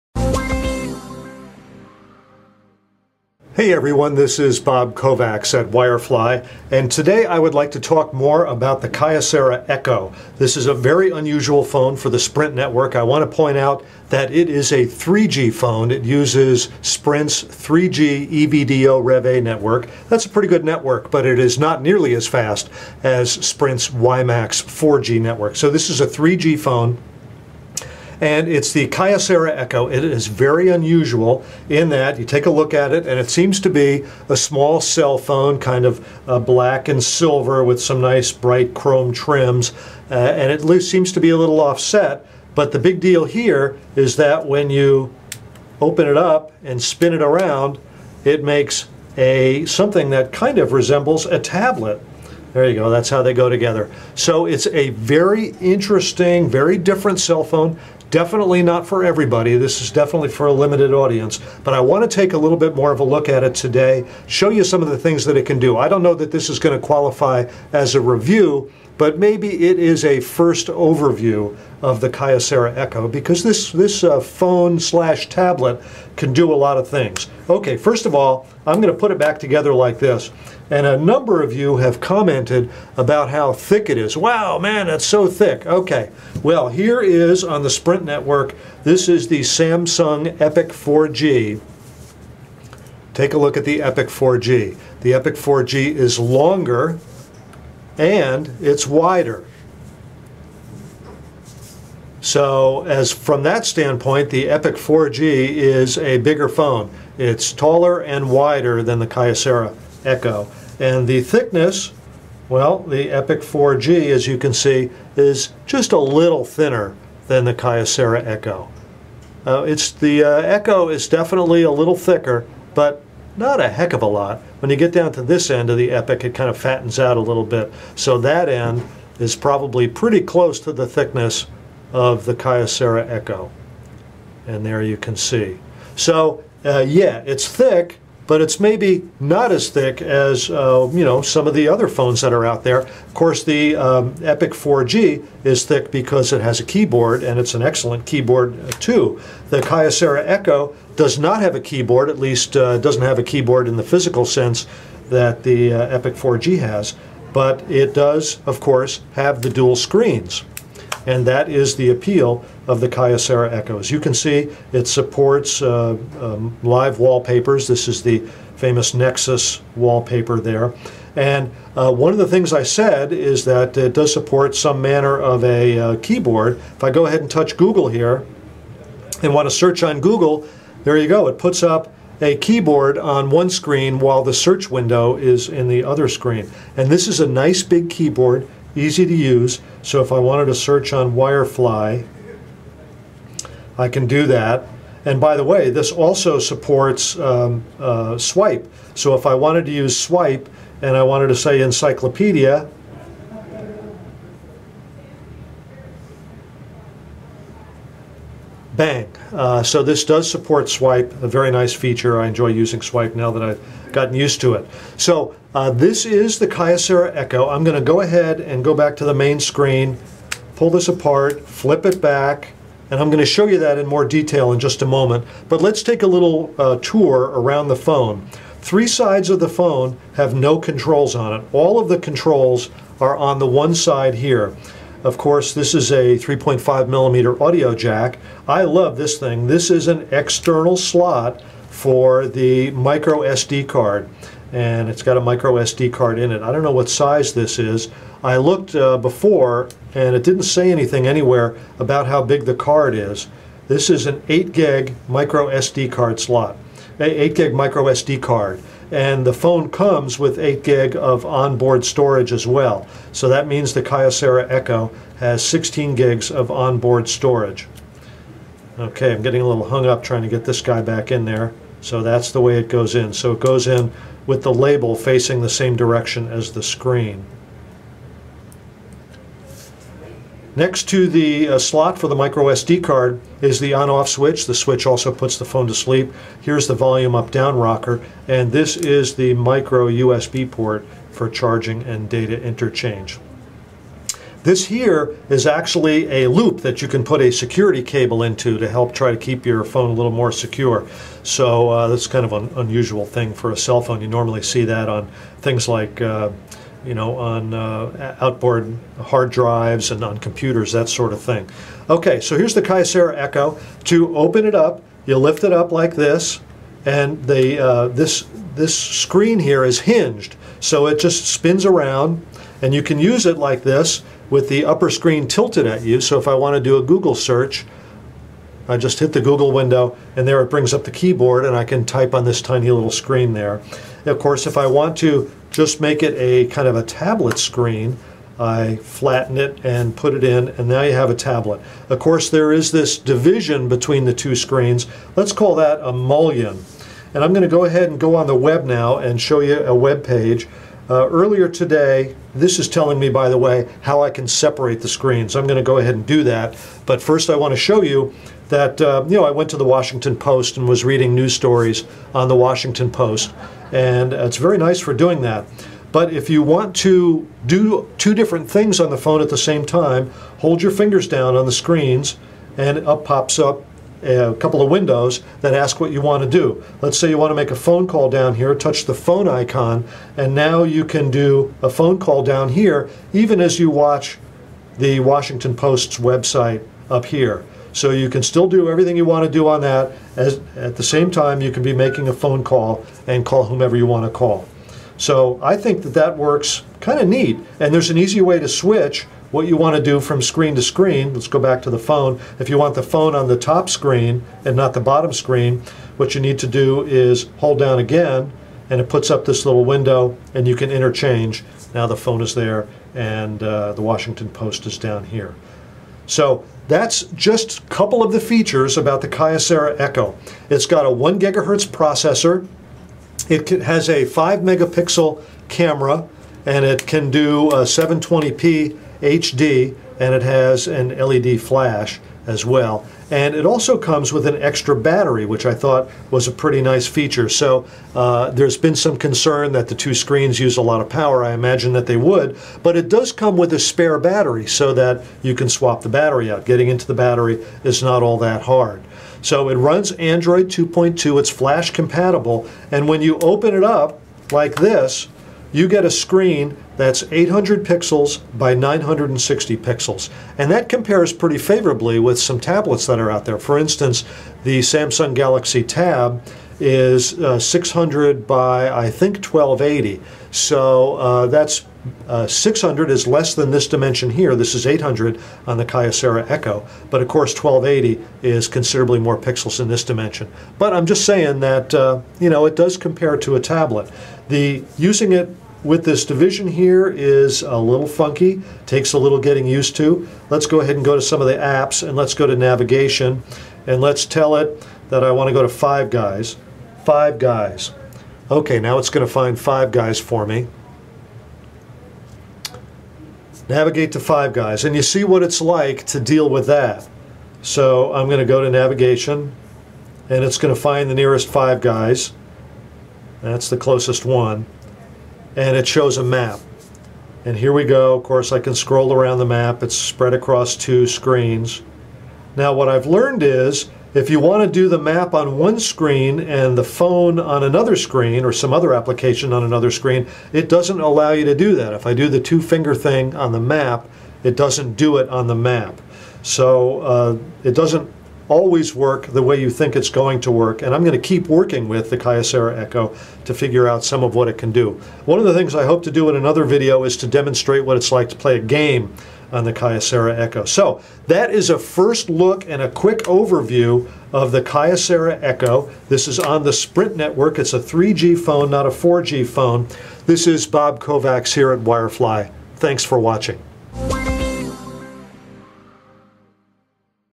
Kyocera Echo Overview
Now that we've had a chance to use the Kyocera Echo for a day, we've learned a few things and wanted to show viewers more about this unusual phone. The Echo has been passed around the office and the results are about 50/50 -- half the people think it's just too strange and the other half think it has possibilities. A couple people even made noises about getting one. So take a look for yourself and decide if the Kyocera Echo meets your needs for a cell phone.